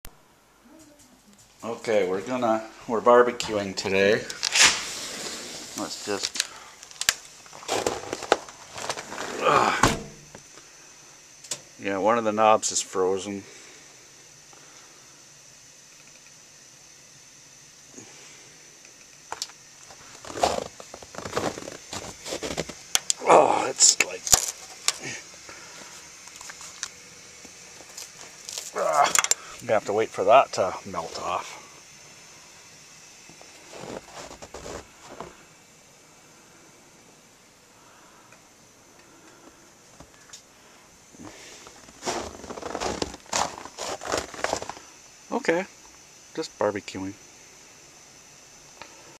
Winter BBQing
We're BBQ today for St. Valentines Day here in Kilbride, ON.

The BBQ is a bit frozen, we can only turn 2 out of 3 burners on.